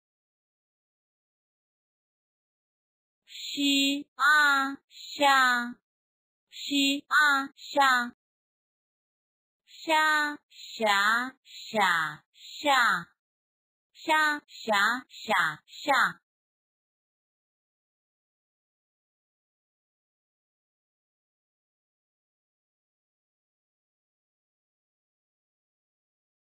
Chinese Pinyin Practice - sha
List of exemplary Chinese words with retroflex tune sha